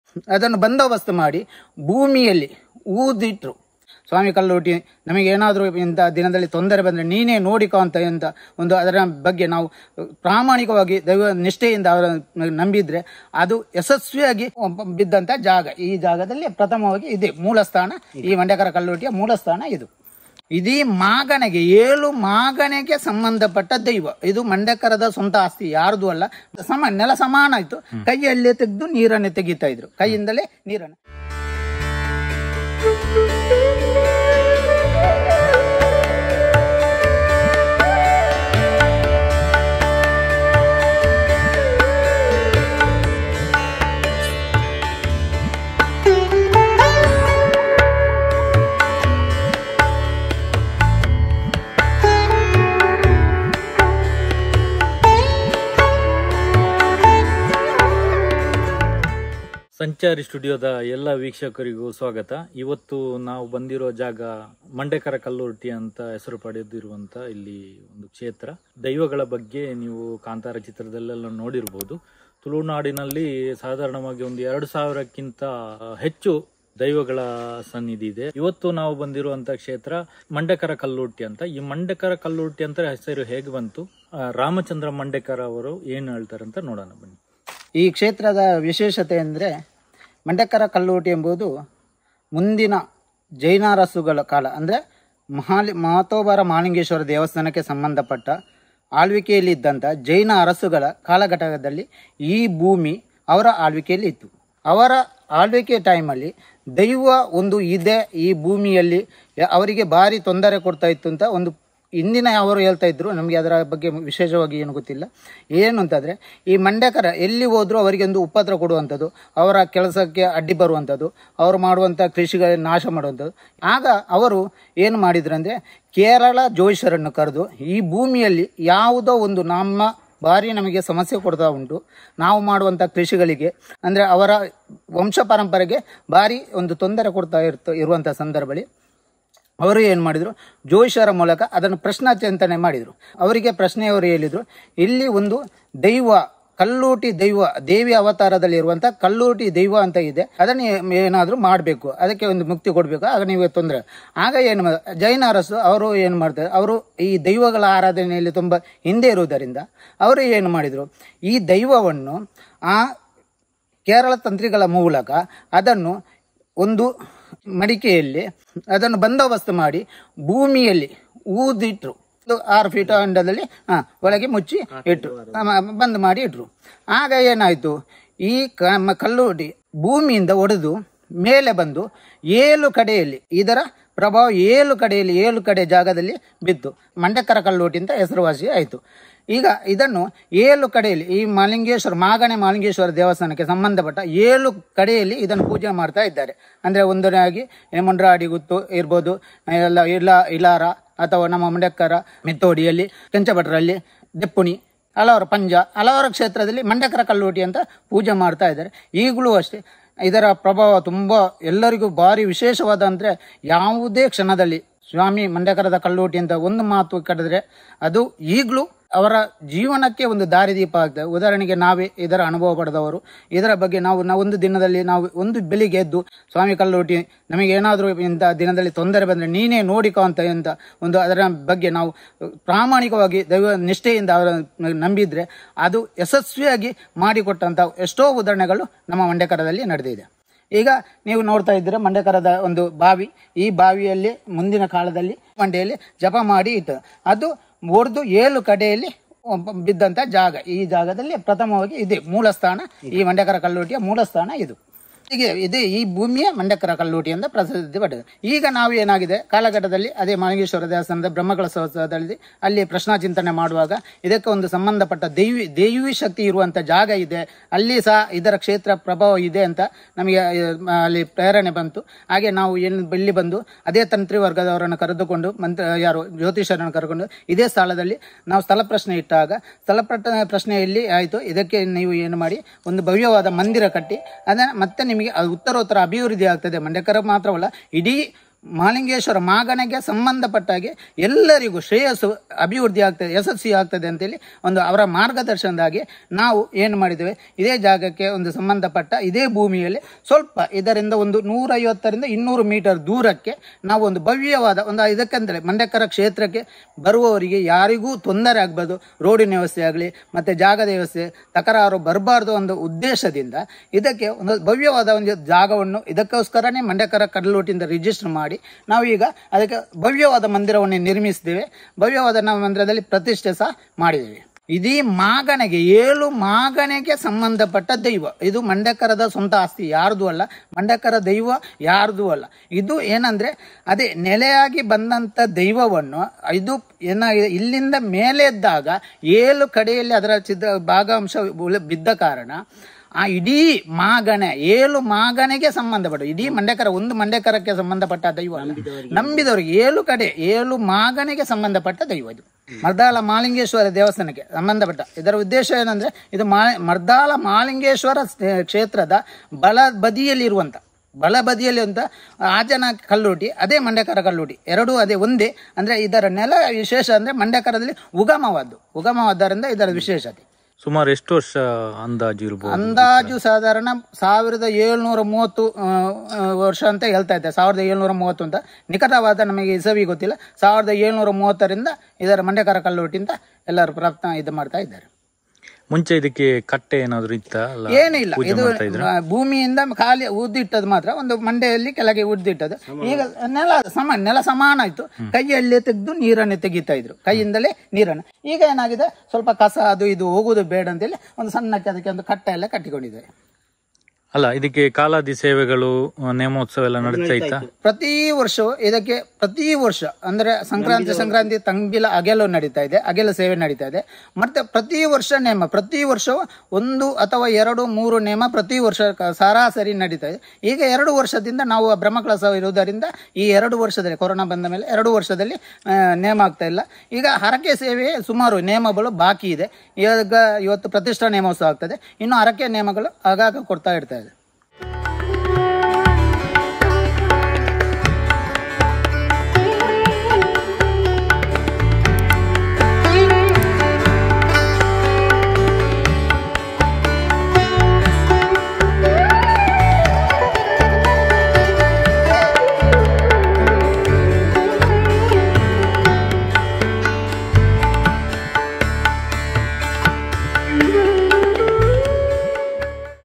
ಮೈಲಿಗೆಯಾದವರು ಹತ್ತಿರ ಹೋದರೆ ಬಾವಿ ನೀರು ಹುಳ | ಉದ್ಭವವಾದ ಬಾವಿ | ಶ್ರೀ ಕ್ಷೇತ್ರ ಮಂಡೆಕರ ಕಲ್ಲುರ್ಟಿ ದೈವದ ಕಥೆ
Thanks for Visit ‘’SANCHARI STUDIO’’